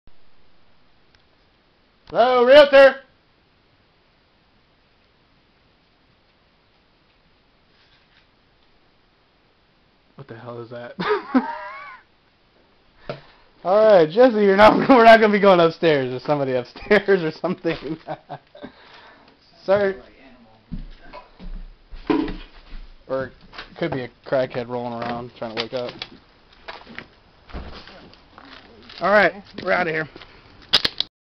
Chased out by a crack head ??? SUBSCRIBE FOR MORE WILD REAL ESTATE
...JJ and Andrew set forth into a vacant home which has sat empty for 7 years to take pictures for an out of state client who is wanting to purchase the property... to add onto that long sit, Andrew comes to find out that the property has been sitting this whole time with NO LOCKBOX!!! The agent tells him that the backdoor (which is hidden by bushes the size of Mount Rushmore) is unlocked and go ahead on in!.. well... To say the lease, pictures of the 2nd floor's property were not able to be takin.! VIEW VIDEO!.
Next time we will make sure to have a HD camera which can catch our c

*footage caught by samsung TL100 Digital Camera*

Think we should spin a show out of this? reply below.. COMMENT ALSO!. Let's hear your thoughts.